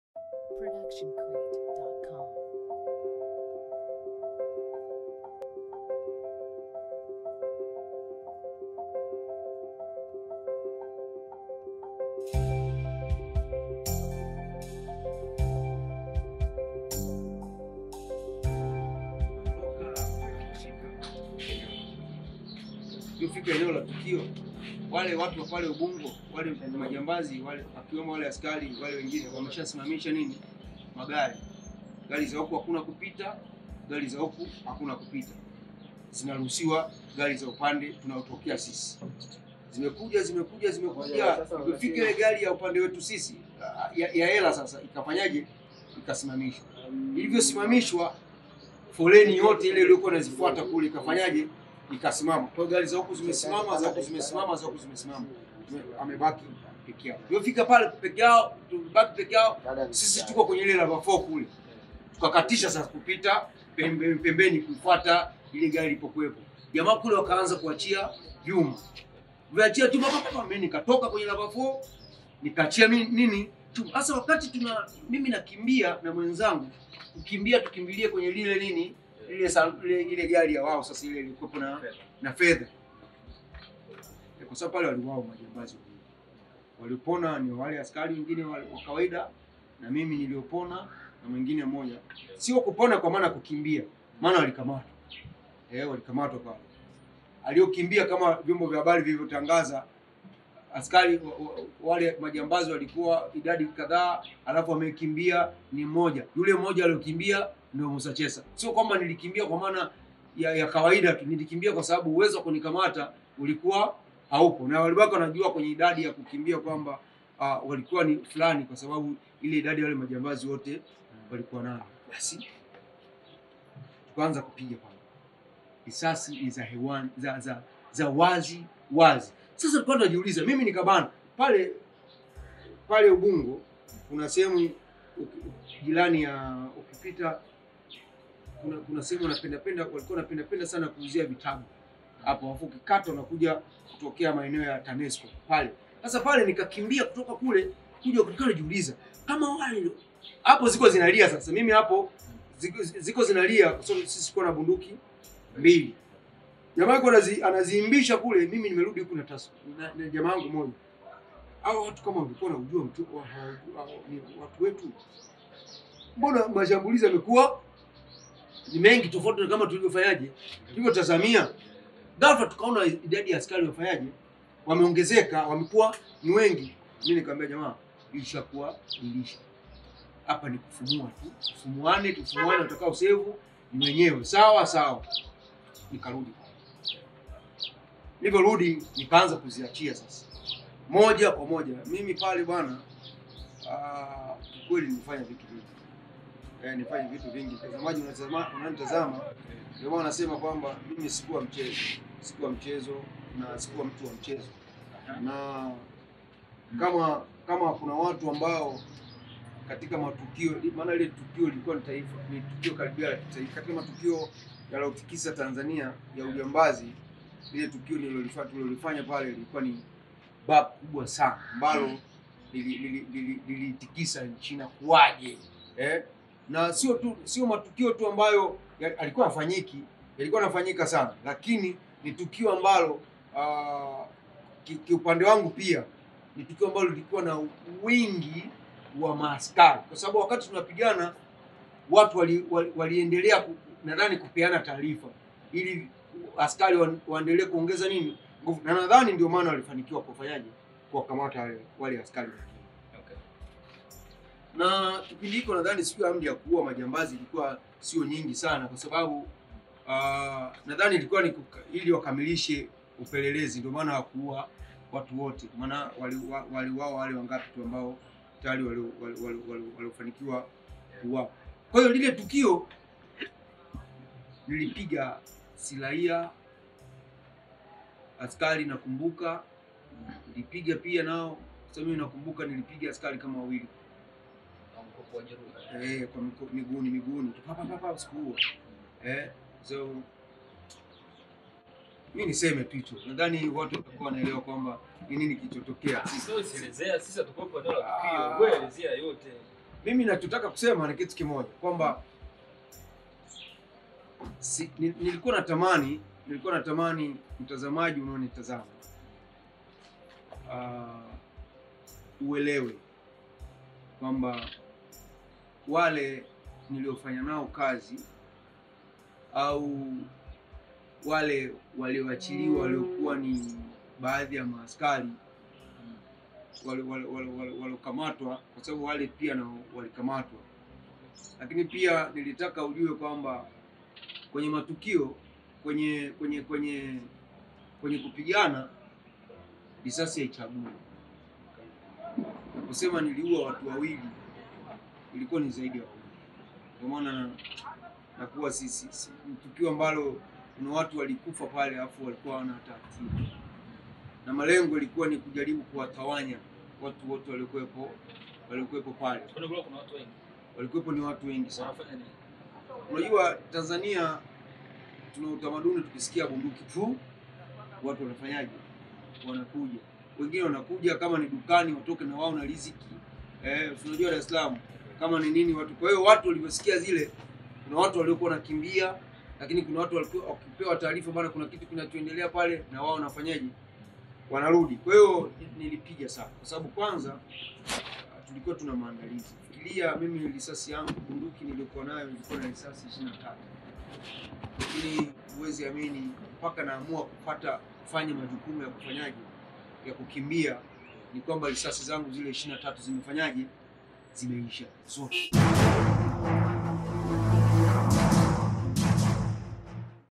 Mussa Chessa aleza chanzo kilichomfanya kuwa jambazi. baada ya kua na maisha magum
history za UJAMBAZI